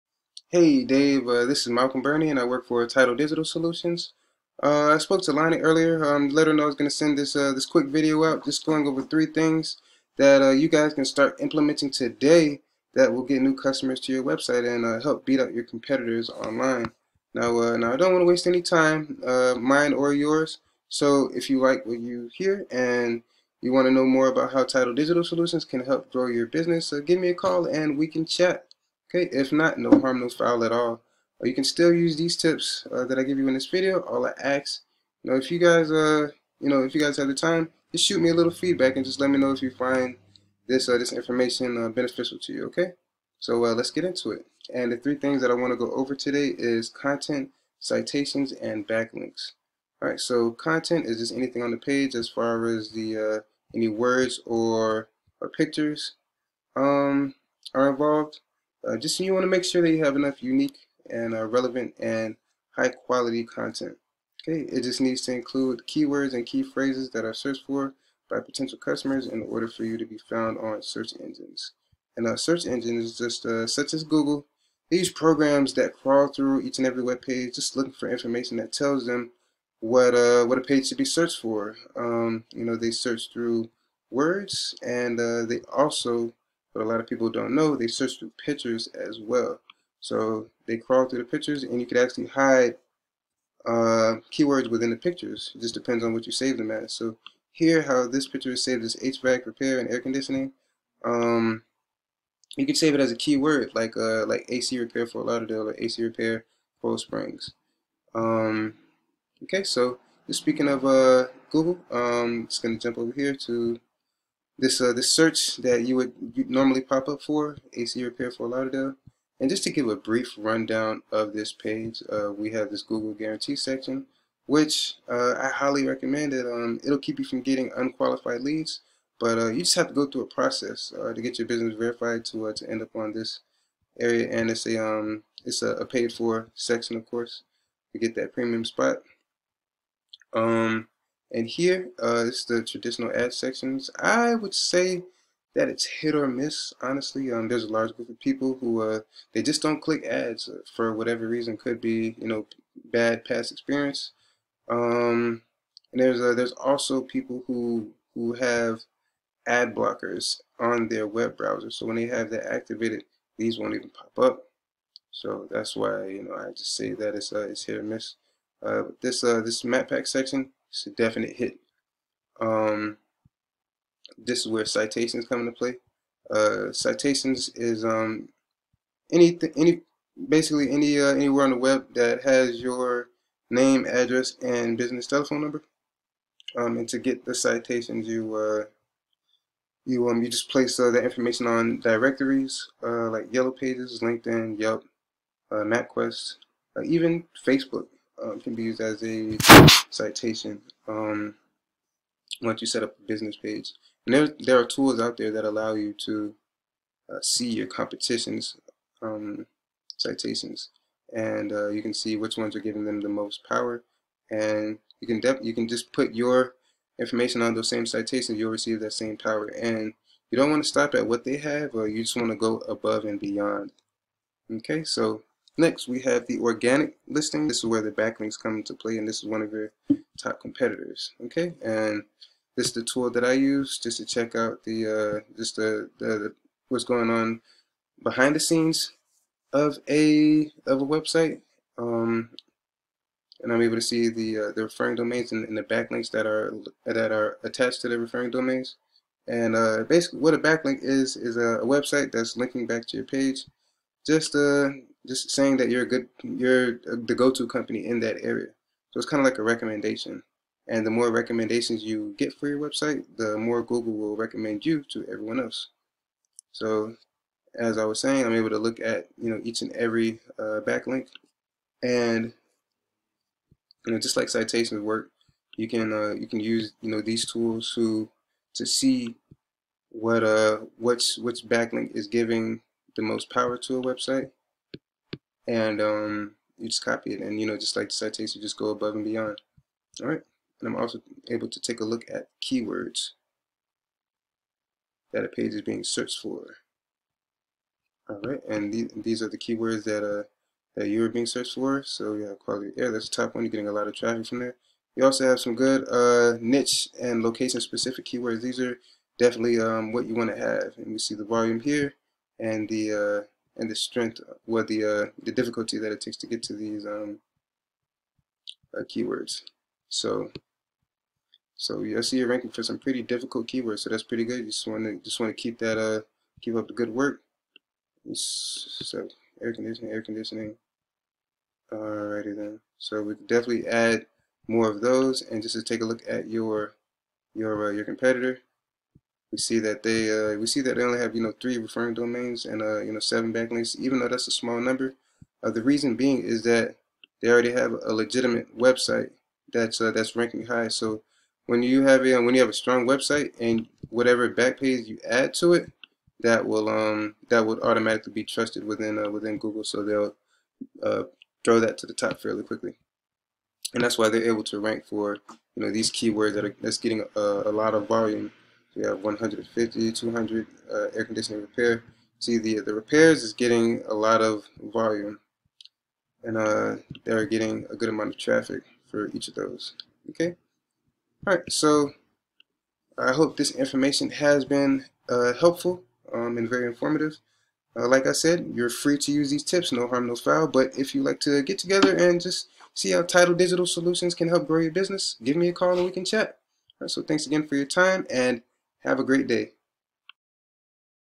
Quality AC vs Competitor
Malcolm Burney shows how 3 simple things can help a company gain customers online. Content, Citations and Backlinks.